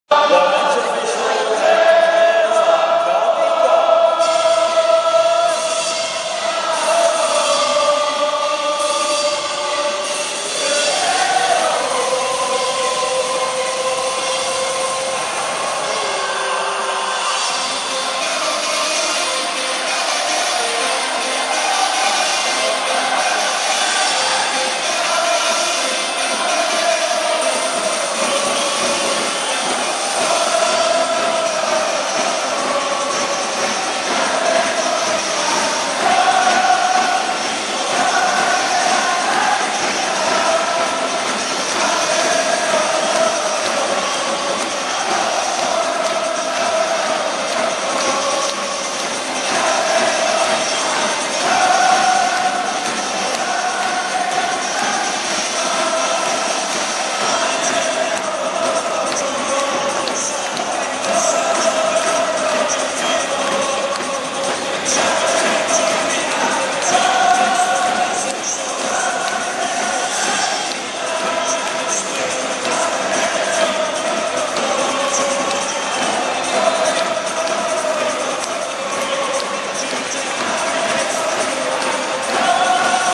2019.08.17 FC東京vs広島 選手入場
明治安田生命J1リーグ第23節　味の素スタジアム